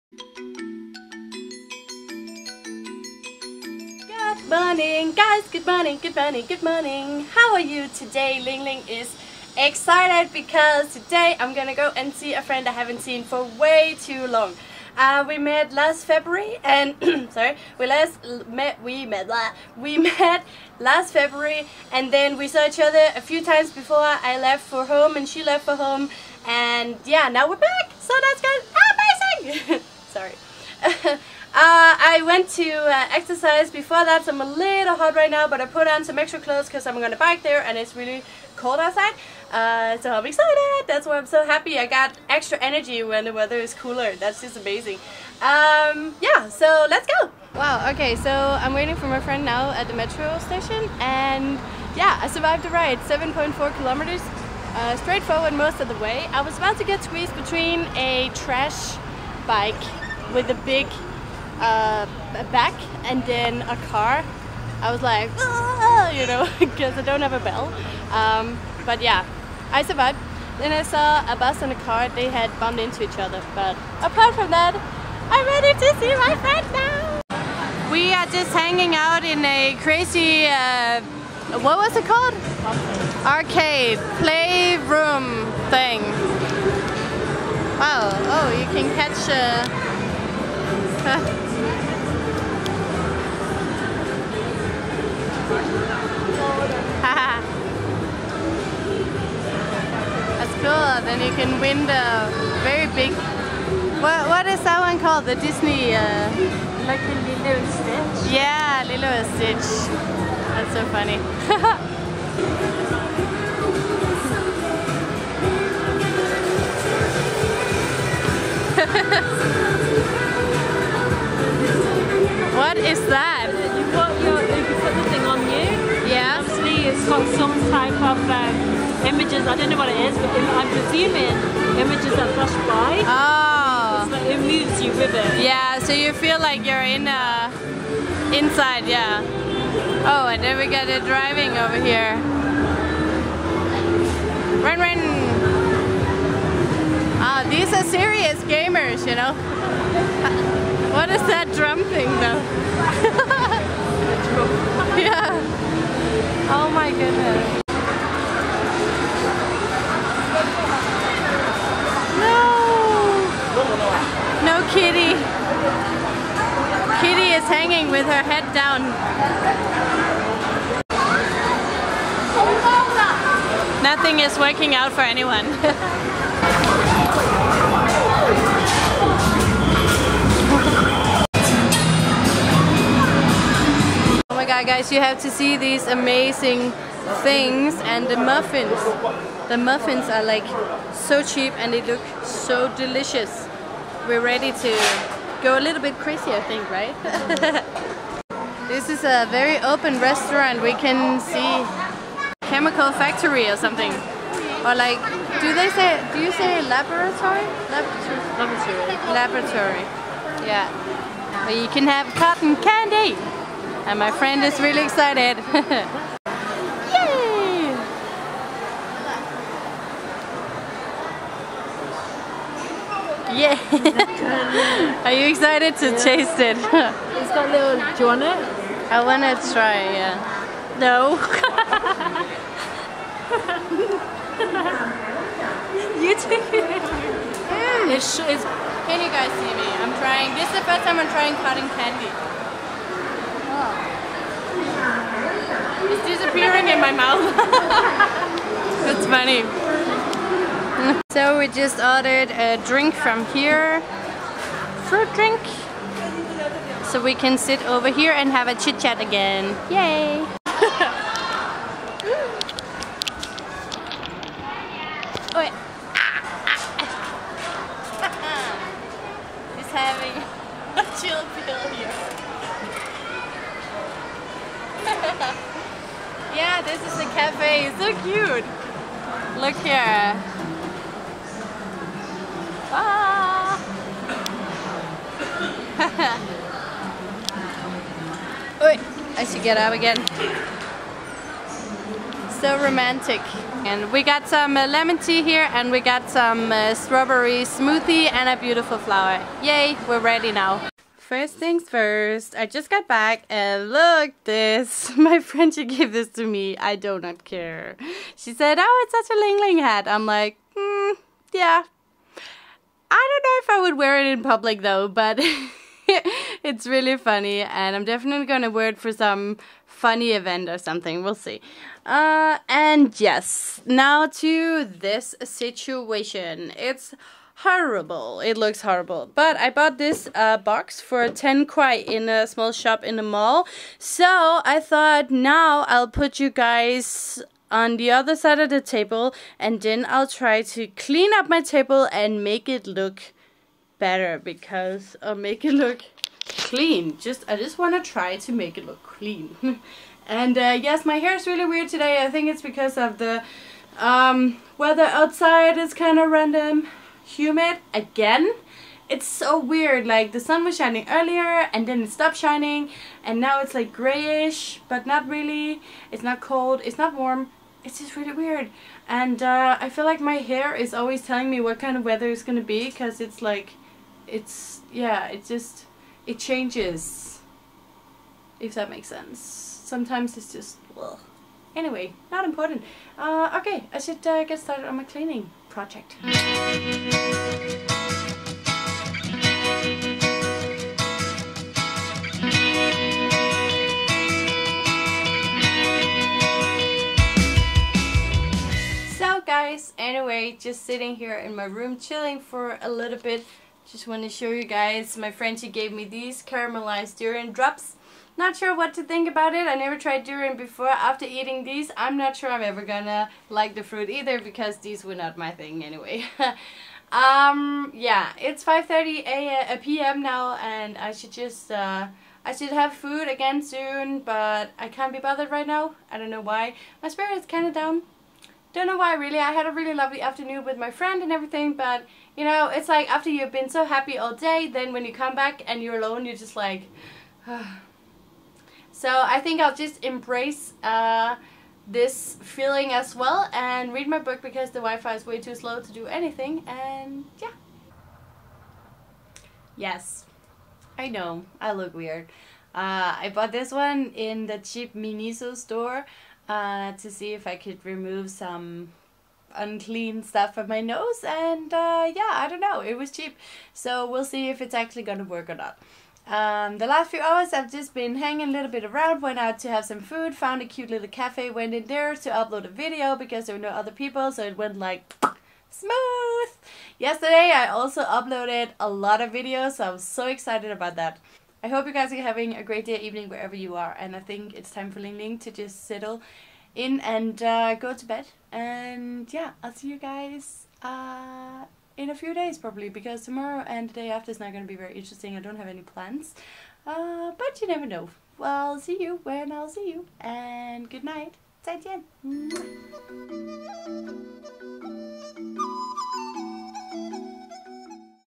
LIFE VLOG CHINA: Eating Candy Floss and Game Adventure //中国生活：吃棉花糖还有玩游戏
Hey guys!

Filmed on the fifth of October :)
I met my long lost friend and we went on adventure in the mall haha

Lingling